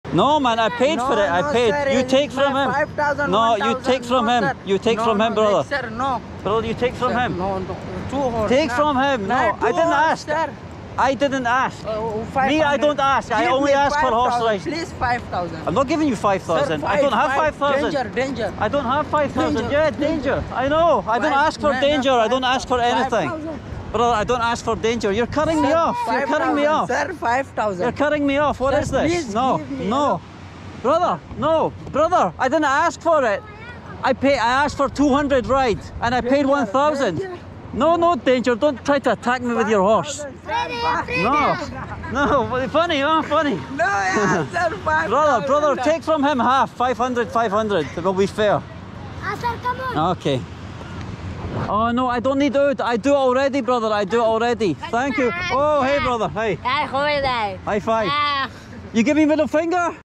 Scammer Demands 5,000 Rupees for Horse Ride in Karachi, Pakistan 🇵🇰
I took a horse ride at Seaview Beach in Karachi, but the horseman handed my horse to his brother who dragged us on a long ride, and wouldn't stop when I told him to let me off.

After returning me to the original horseman he demanded 5,000 rupees rather than the 200 we had agreed.